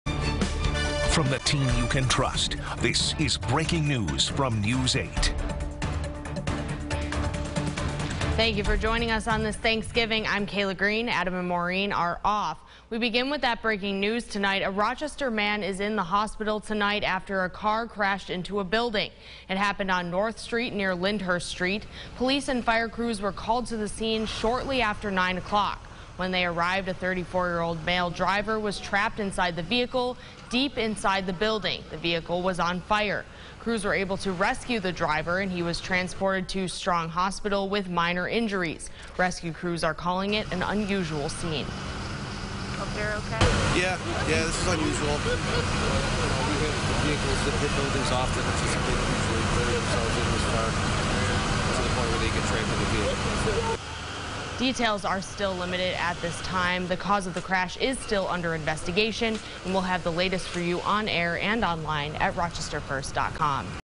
One hospitalized after crashing vehicle into North Street building
ROCHESTER, N.Y. (WROC) — Officials say a 32-year-old city resident is in the hospital after crashing into a building on North Street and Lyndhurst Street Thursday night.

The car initially caught fire upon impact but it was quickly put out. The driver and sole occupant of the vehicle was trapped and then extricated and taken to Strong. They say he has minor injuries.

There was no one in the building at the time of the crash. The investigation is ongoing, but there are no charges at this time.